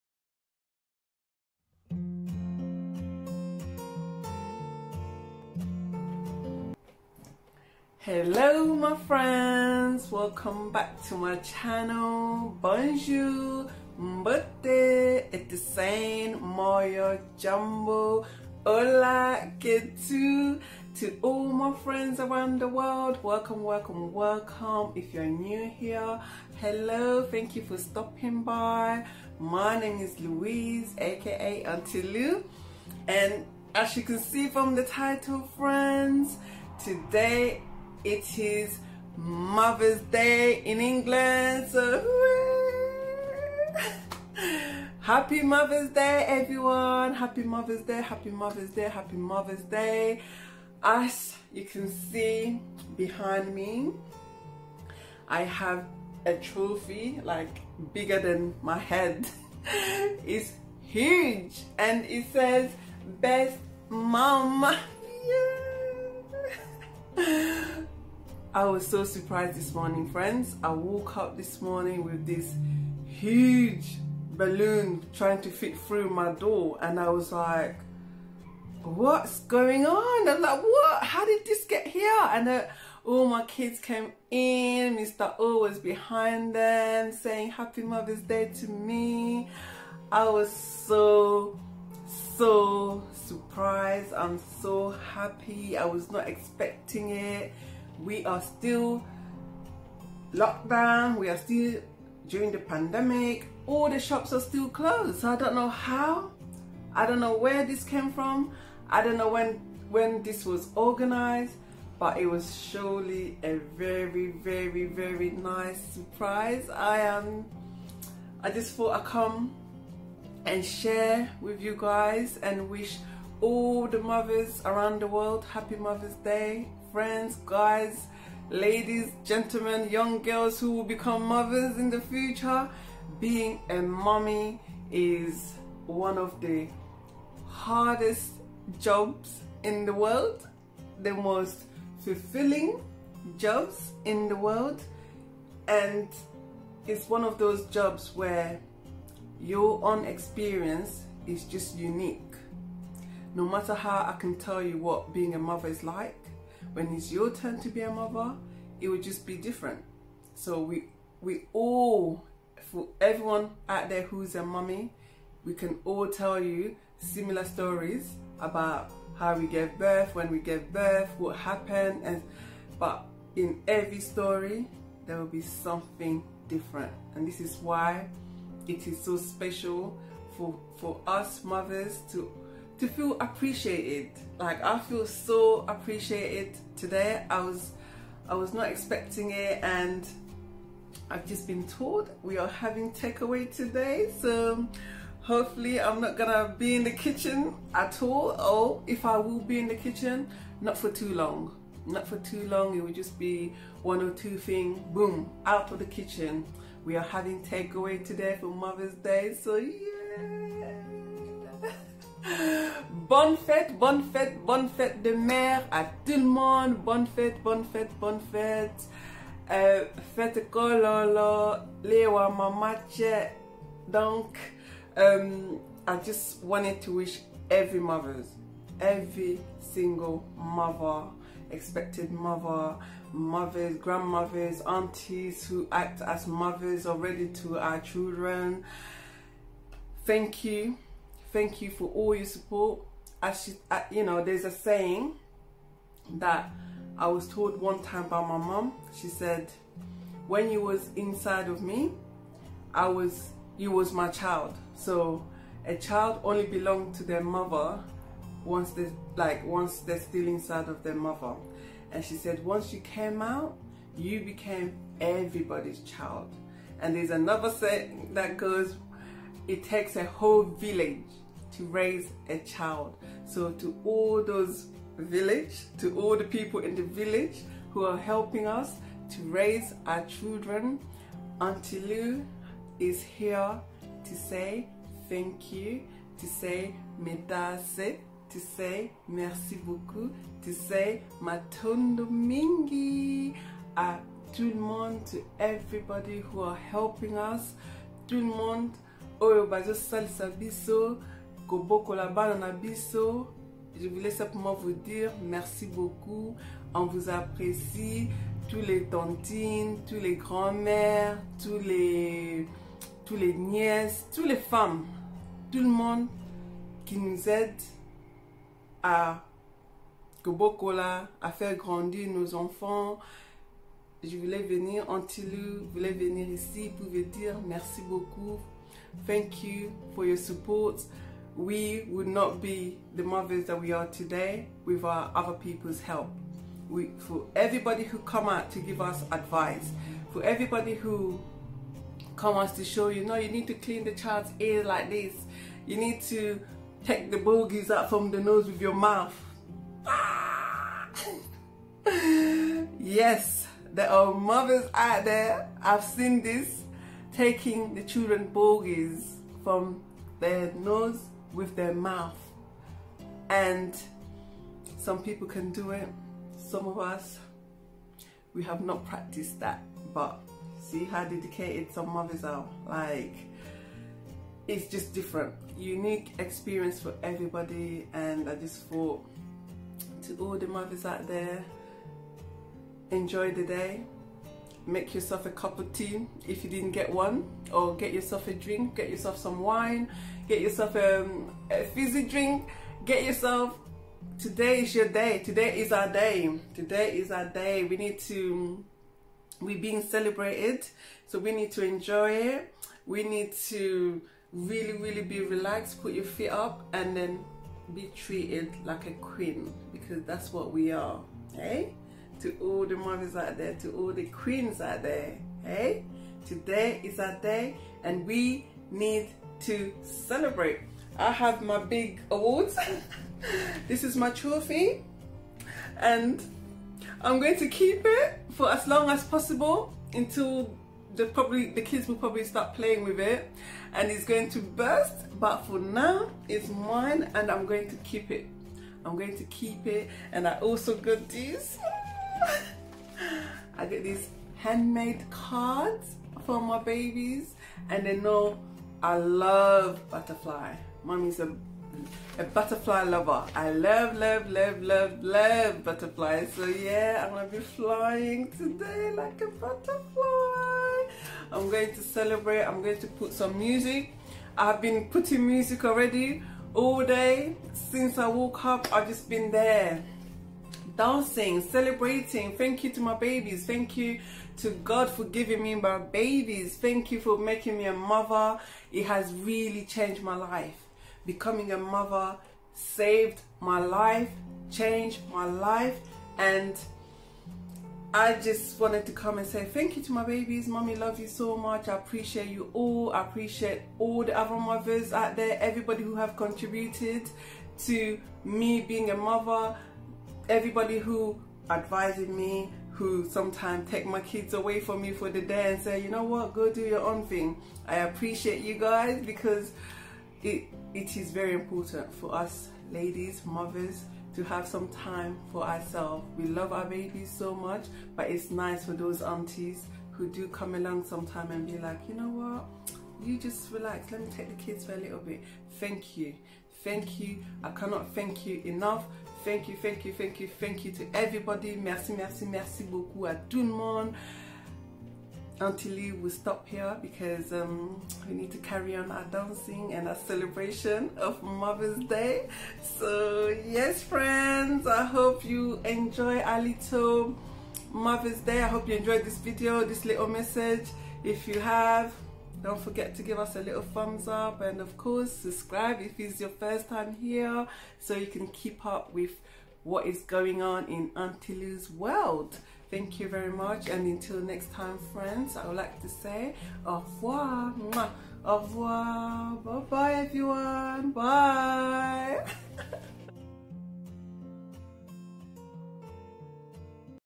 Happy mother's day
To all the women who are mothers around the world. I just wanted to come and wish you a happy mother's day. Thank you so much for all your support and encouraging words.
Aunty Lou love and appreciation you.